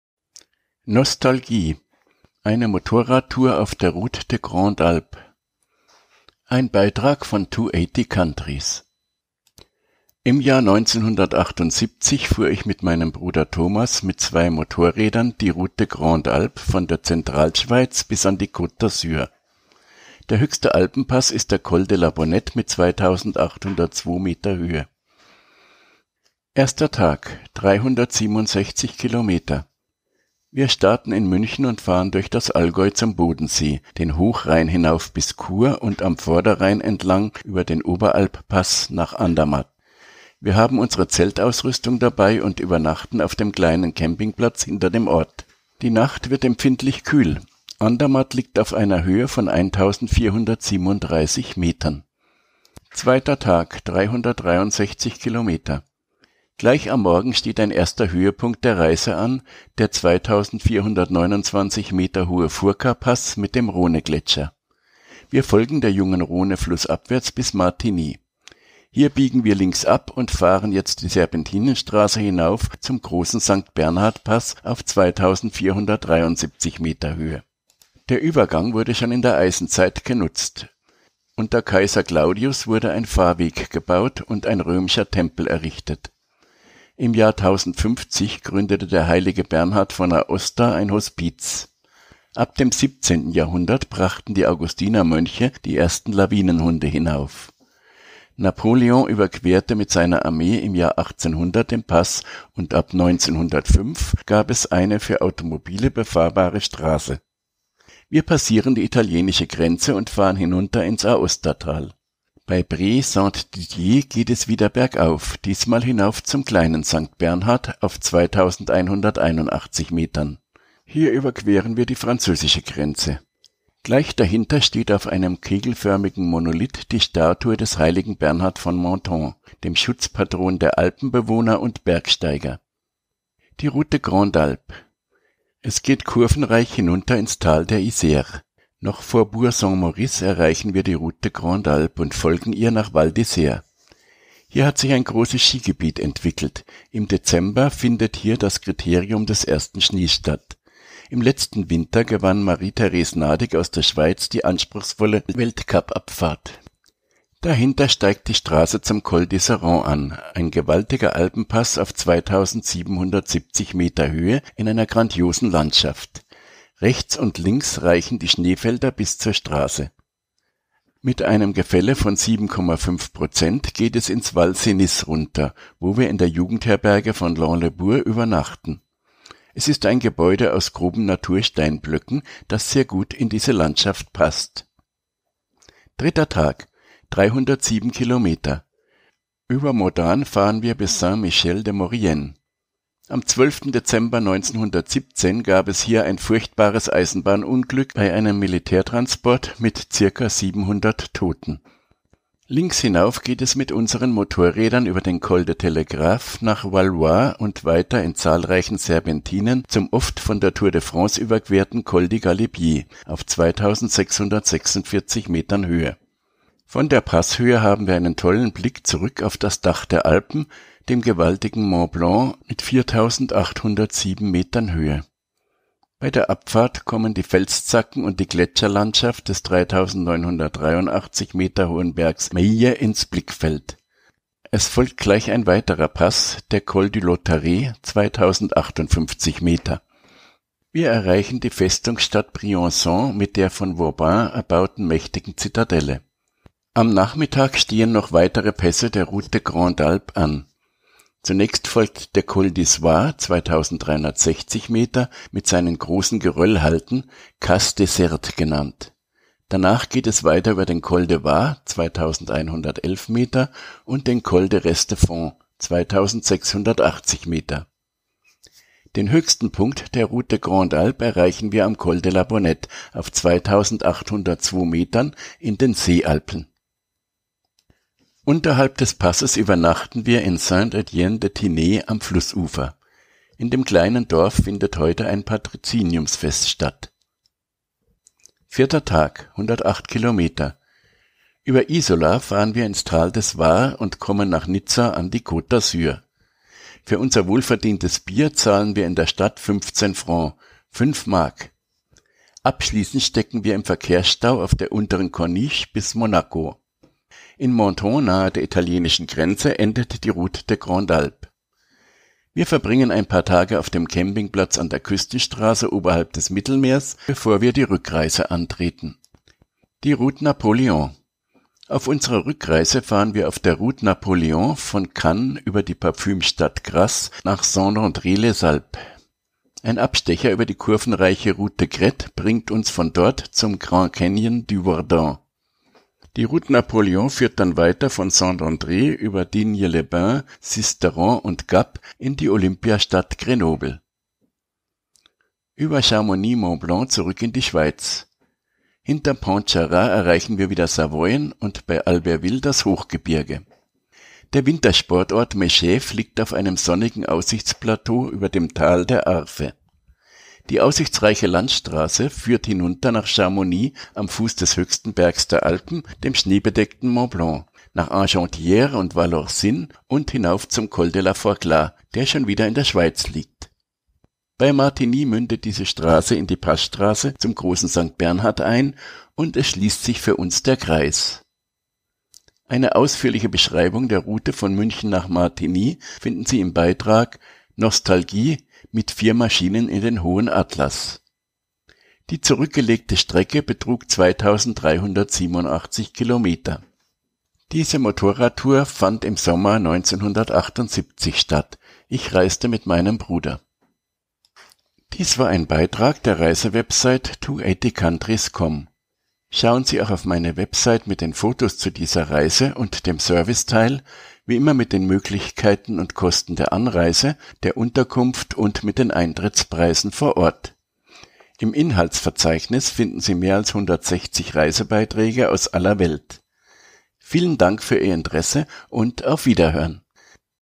Nostalgie – Motorradtour auf der Route des Grandes Alpes - 1978 [DE]
Im Jahr 1978 fuhr ich mit meinem Bruder Thomas mit zwei Motorrädern die Route-des-Grandes-Alpes von der Zentralschweiz bis an die Côte d`Azur. Der höchste Alpenpass ist der Col de la Bonette mit 2.802 Meter Höhe. [Podcast][Fotogalerie]

Die Fotos wurden noch nicht digital aufgenommen.